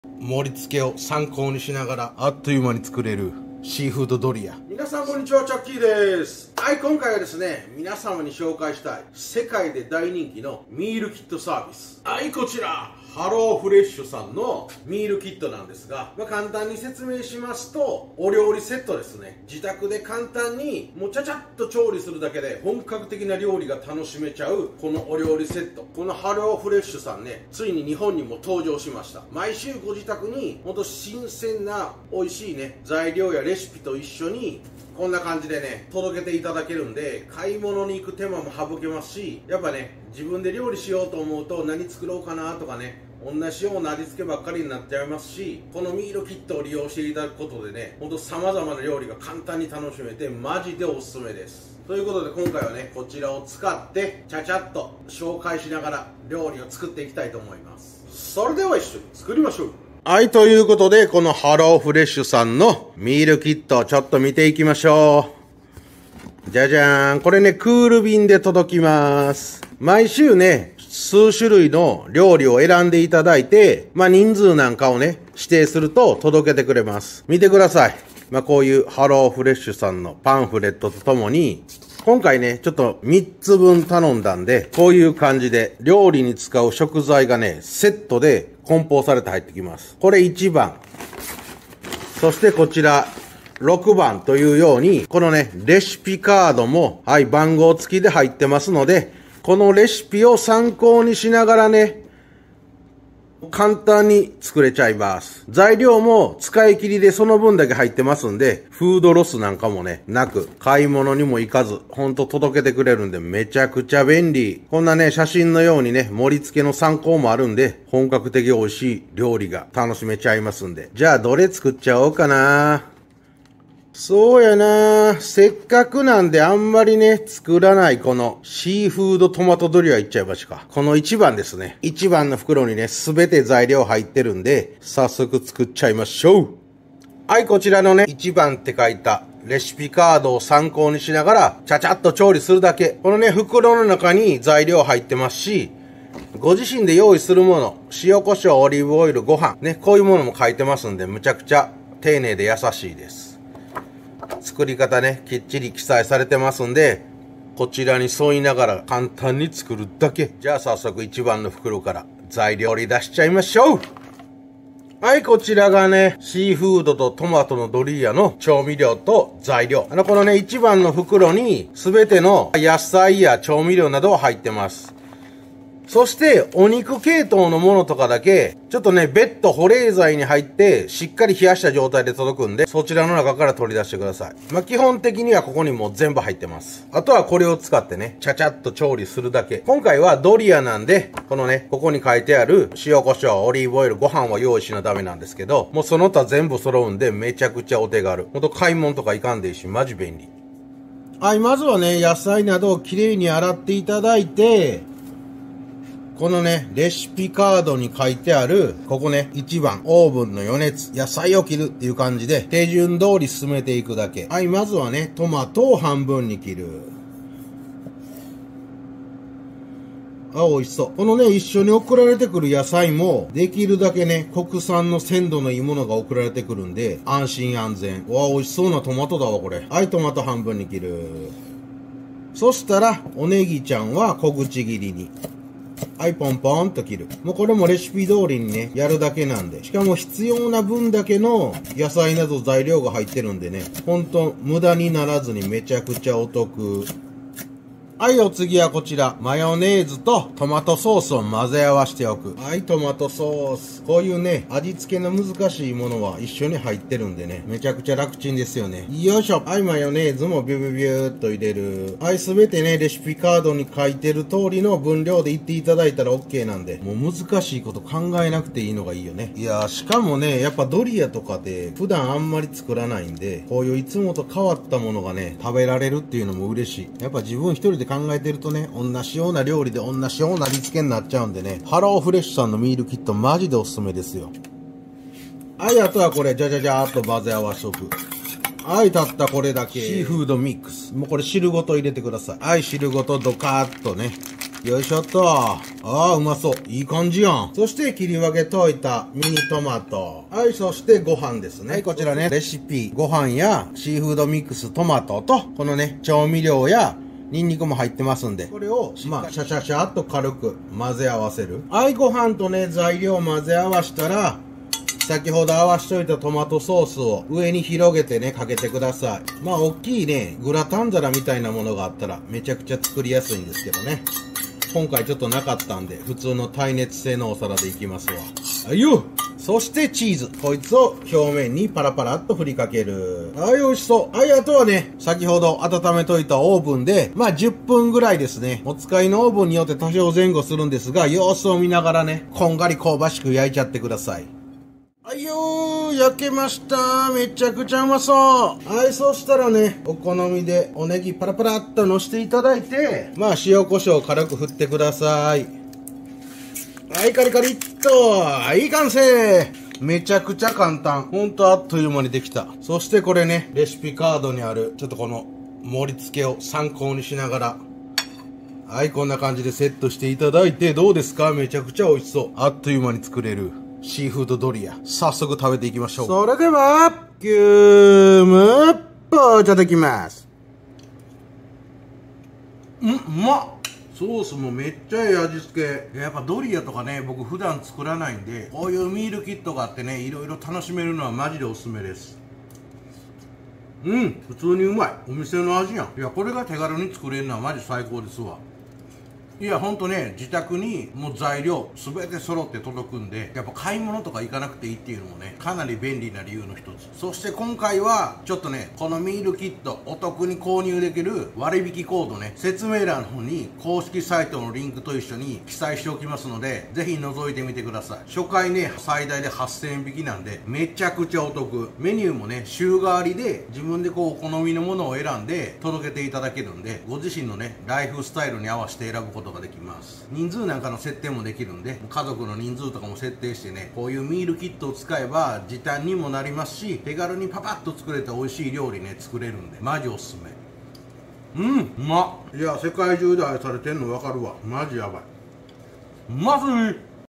献立を考えるのやめました!?お手軽ミールキットで毎日飽きない美味しいご飯作り…!!
今回は、世界17カ国でサービス展開している
HelloFresh（ハローフレッシュ）！
ドイツ発のミールキット宅配サービスのご紹介です

 「POCHA 」 (初回注文最初の3箱が最大8,000円OFF)

専用リンク
↓↓↓からがお得です

2022年3月時点でも累計販売食数は約10億食に達し
世界中で人気のミールキット！

食材や味付けの調味料など、計量・小分けされ
パッと調理するだけで本格的な料理が作れる
お料理セットです。

週替りで数種類の料理の中から選んで
毎週ご自宅に届けてくれるので買い物にもいかなくてもok、献立を考え無くてもok
めちゃくちゃ便利でお得なサービスです。

【Hello Fresh Japan】↓↓↓公式ページ

@HelloFresh Japan